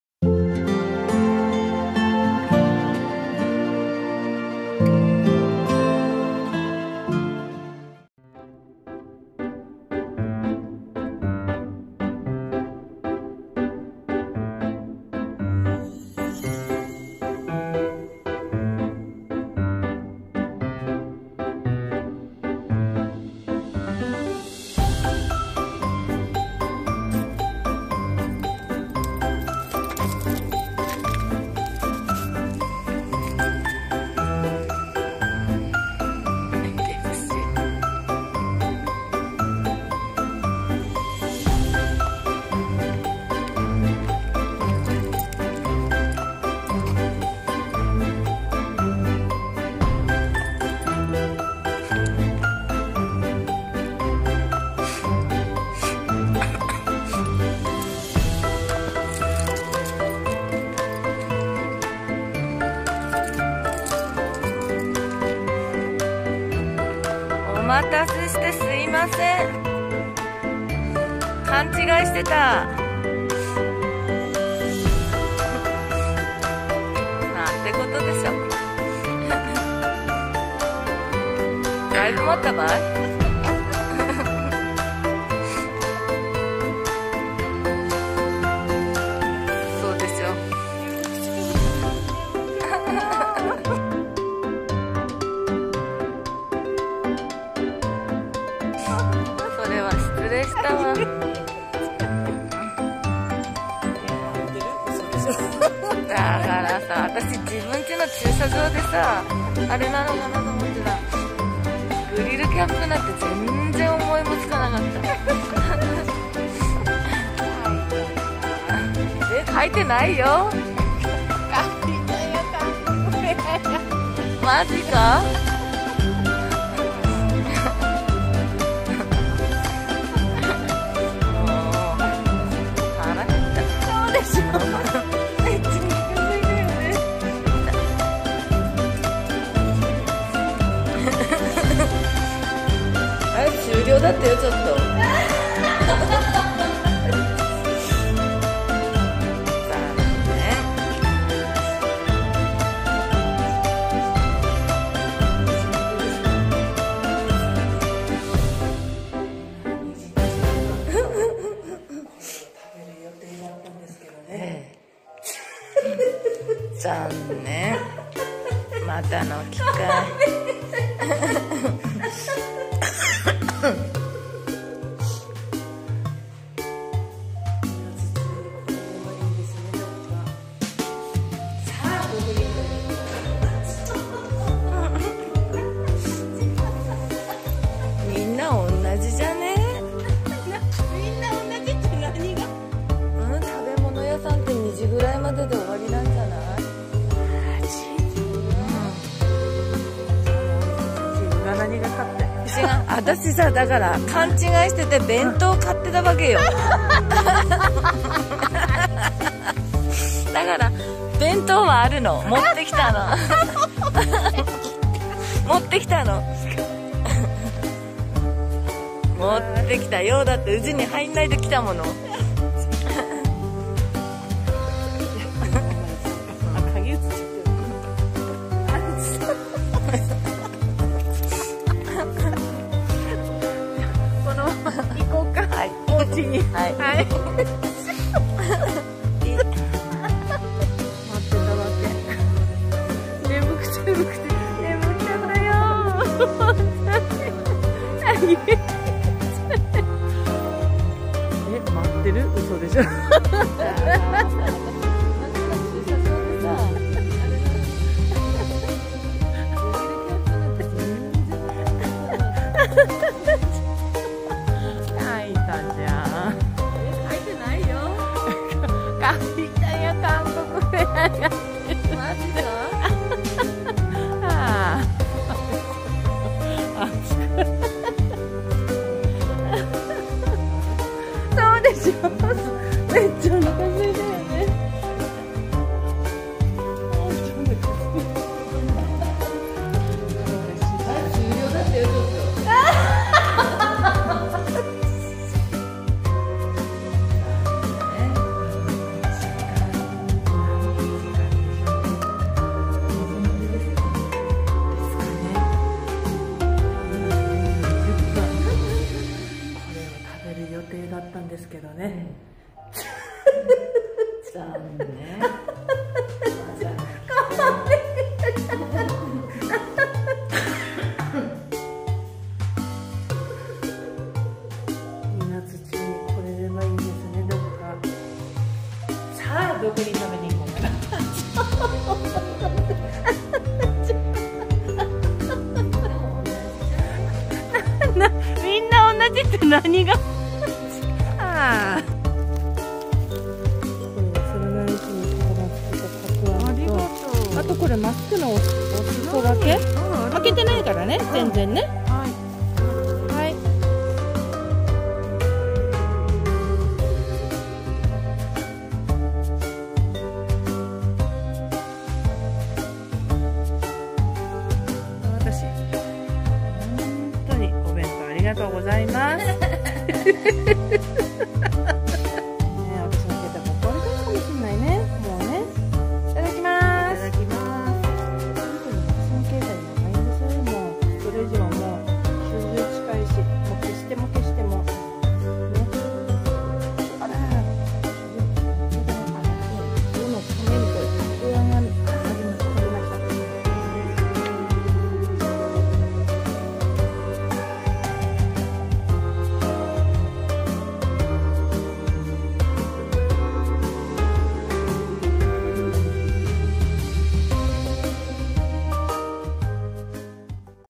【50代主婦の休日】さぁ〜ビュッフェへ行こう
数日前に友達とビュッフェで韓国フェアやってるから
行きたくて誘いました😆🖐️
そして当日、、、
笑笑笑😆😆😆